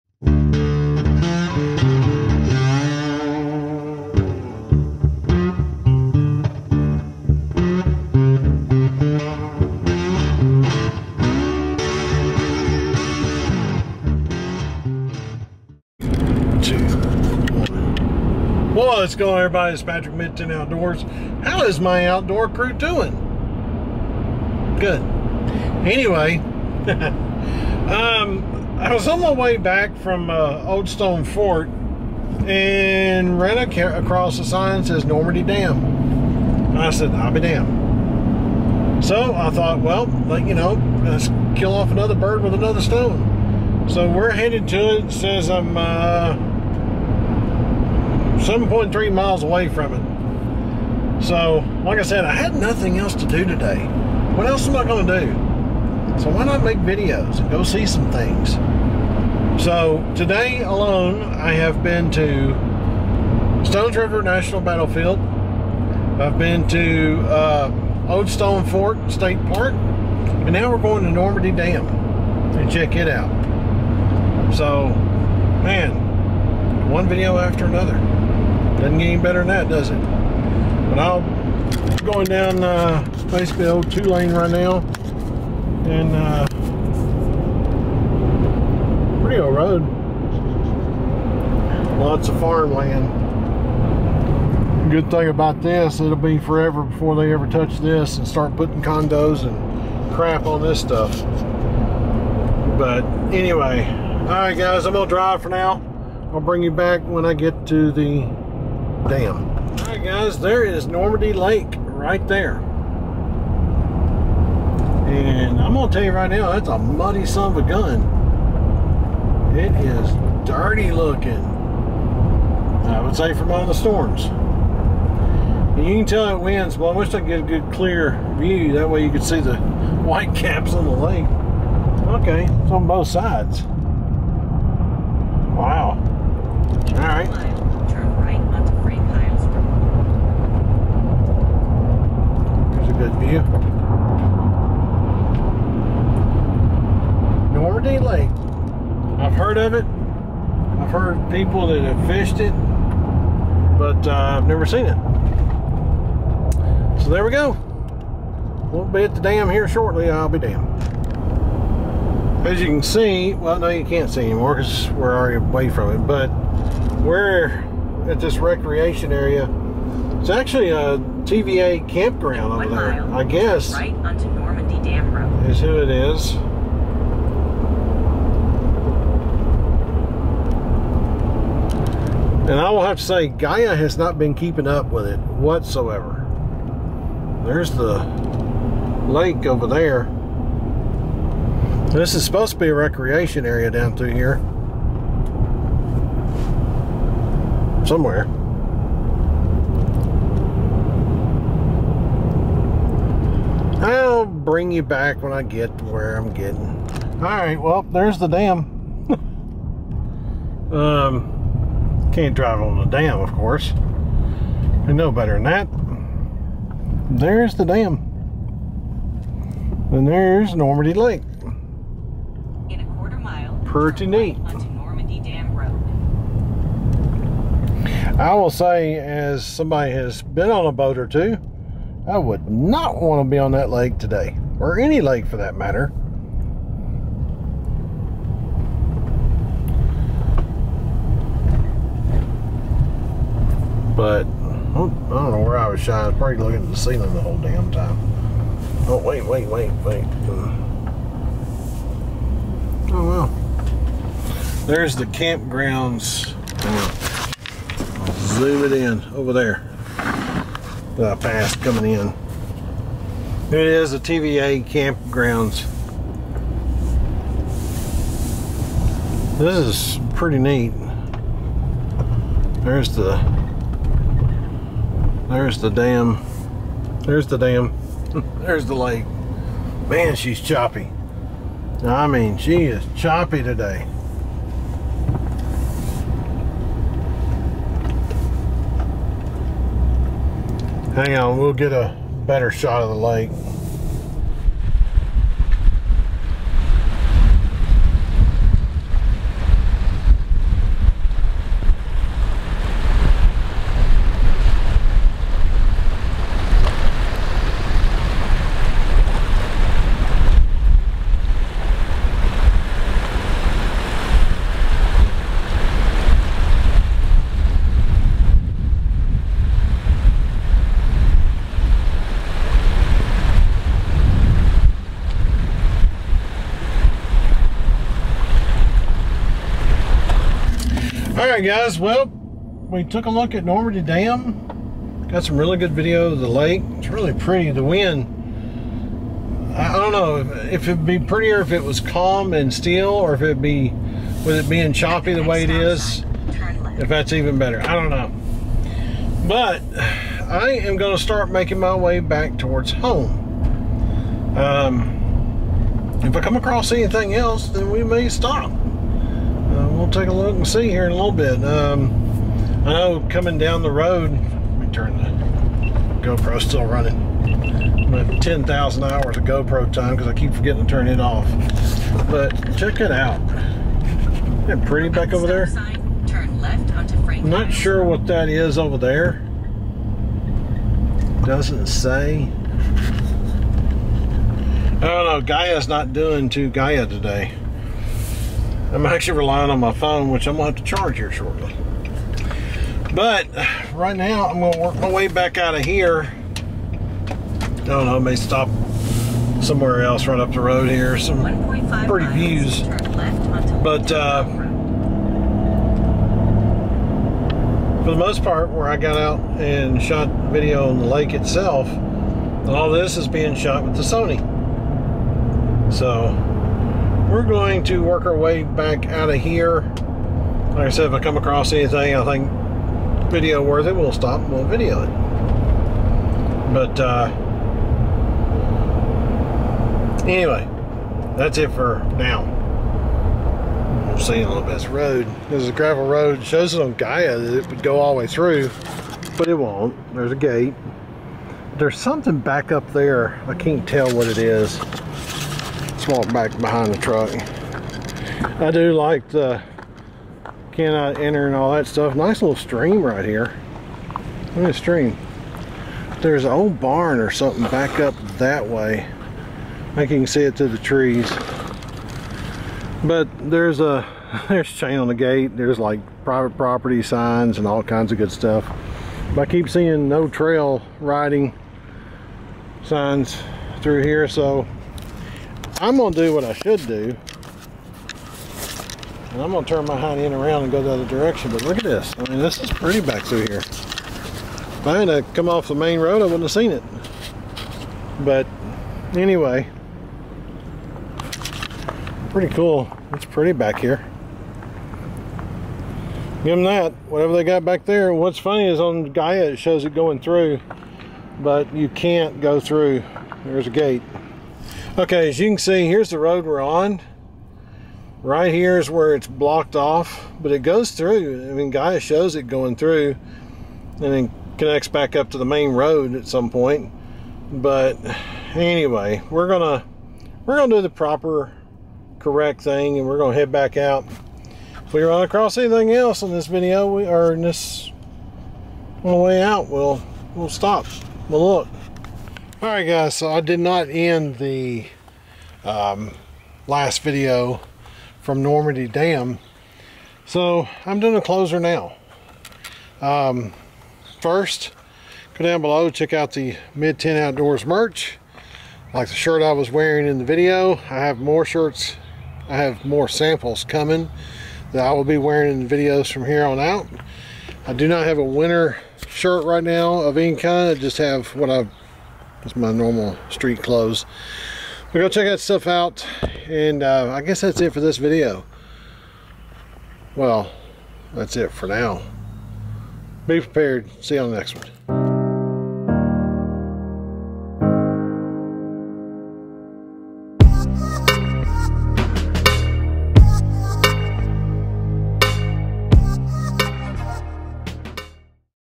Normandy Dam and Normandy Lake
After our other visits on this day, we stumbled across Normandy Dam and Lake on a very windy day.